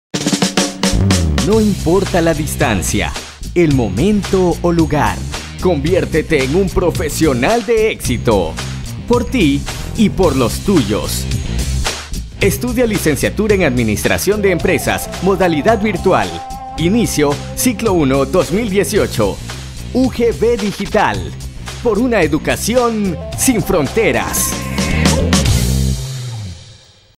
SPOT UGB Digital - Administración de Empresas
Impulsamos una educación sin fronteras, en la cual tú elijes el lugar y el momento para formarte profesionalmente, conoce nuestra carrera Licenciatura en Administración de Empresas en modalidad no presencial.